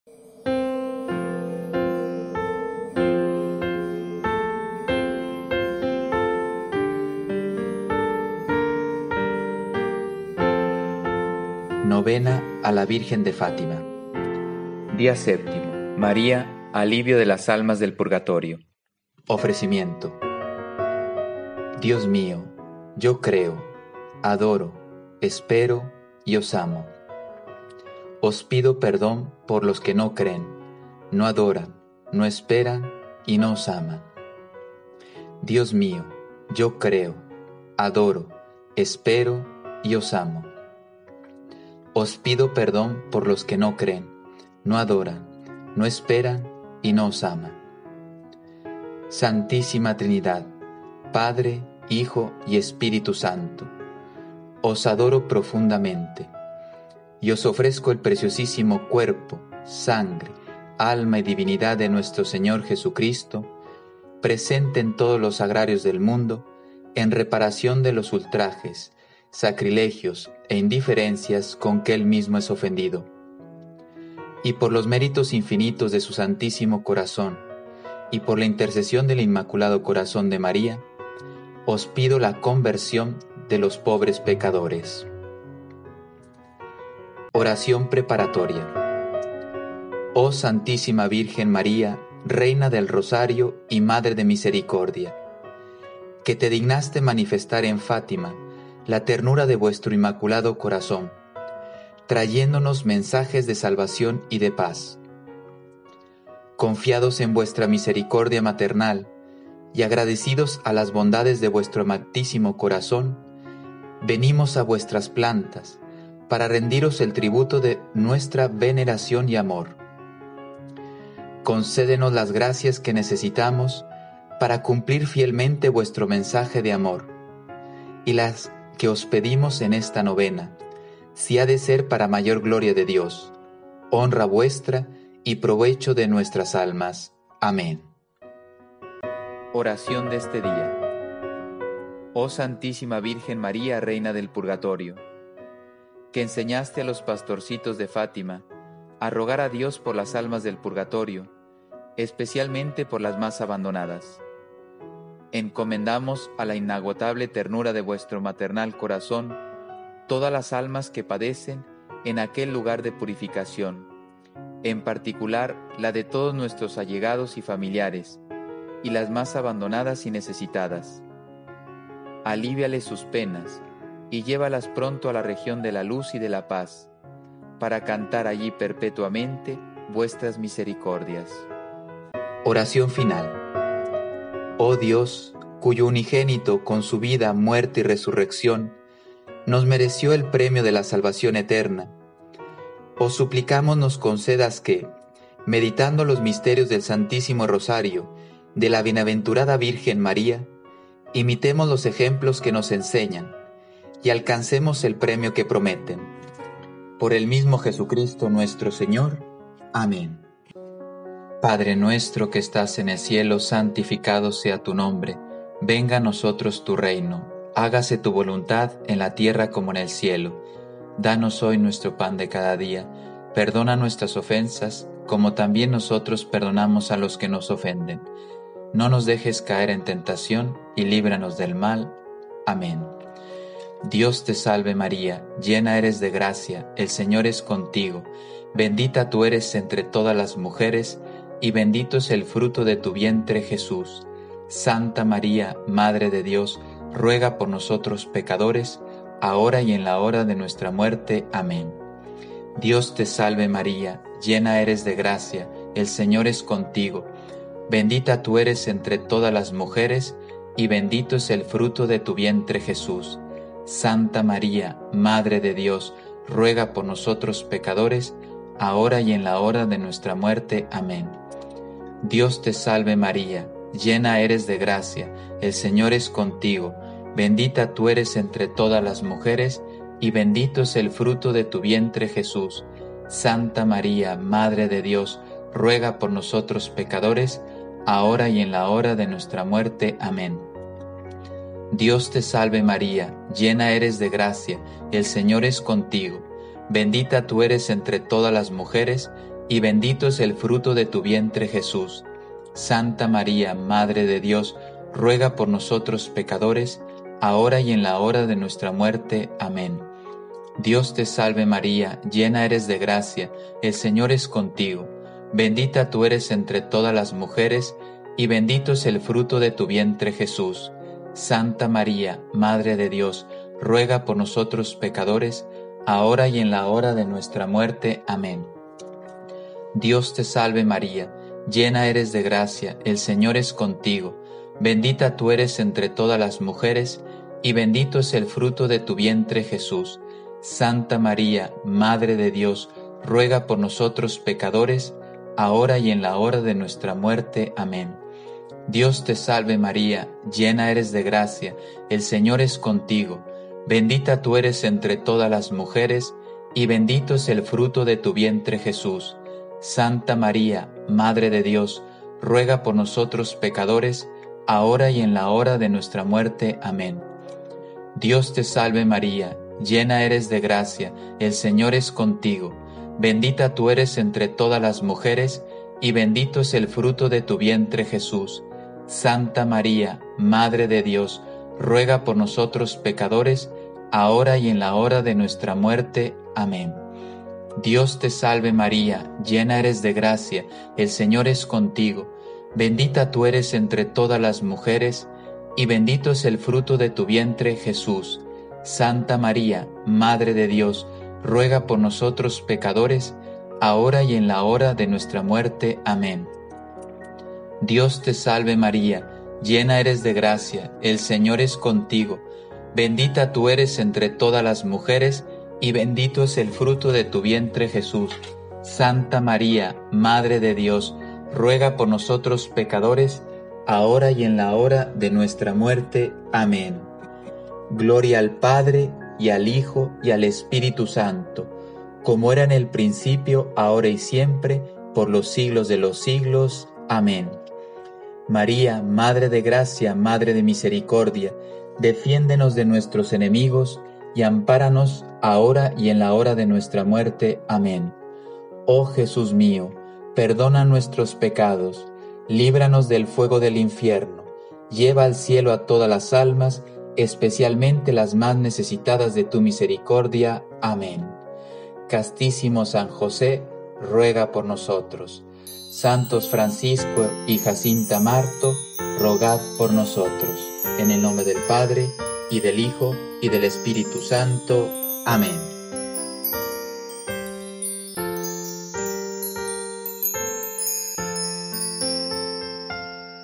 Día 7 - Novena a la Virgen de Fátima: María, Alivio de las Almas del Purgatorio
¡Oh Santísima Virgen María, Reina del purgatorio!, que enseñaste a los pastorcitos de Fátima a rogar a Dios por las almas del purgatorio, especialmente por las más abandonadas. Encomendamos a la inagotable ternura de vuestro maternal Corazón todas las almas que padecen en aquel lugar de purificación, en particular las de todos nuestros allegados y familiares y las más abandonadas y necesitadas; alíviales sus penas y llévalas pronto a la región de la luz y de la paz, para cantar allí perpetuamente vuestras misericordias.